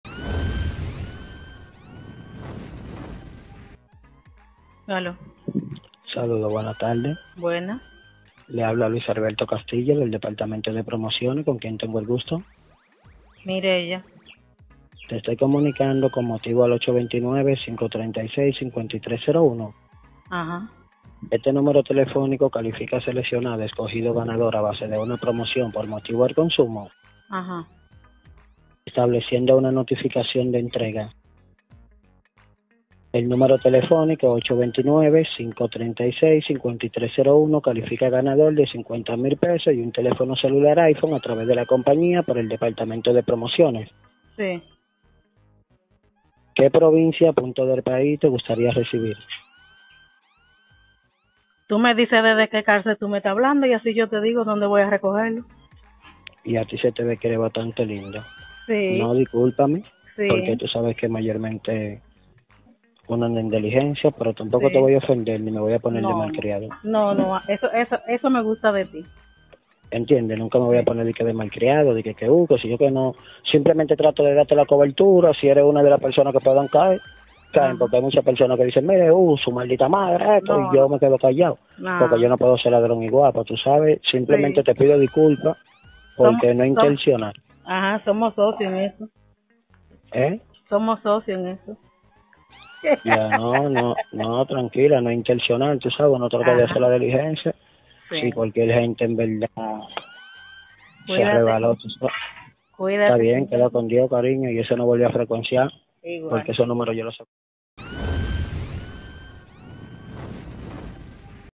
LLAMADA DE UN EMPRENDEDOR | FINAL DE PELÍCULA
LLAMADA DE EMPRENDEDOR PERO DESDE LA PRISIÓN Jajaja, Seguro En Republica Dominicana Te Han Hecho Este Tipo De Llamadas!!!

CUENTAME TU HISTORIA AQUI EN LOS COMENTARIO 🤗🙌😱🤔